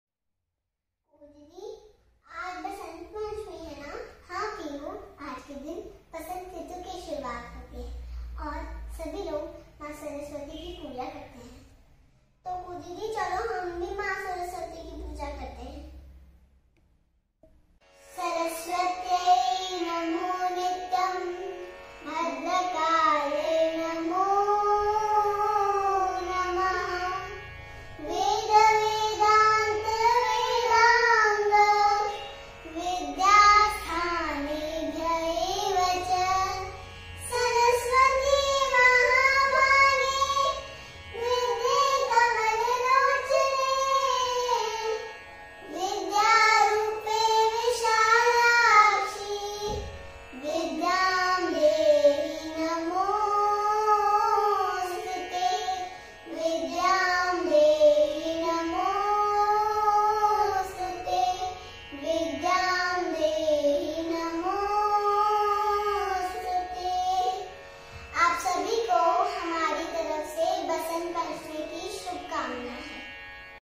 Basant Panchami Vlog# Maa Saraswati Poojan
Basant Panchami Vlog# Maa
Saraswati Poojan

🙏🙏जय मां सरस्वती🙏🙏

Kuhu and Pihu celebrated Basant Panchami by Maa Saraswati Poojan..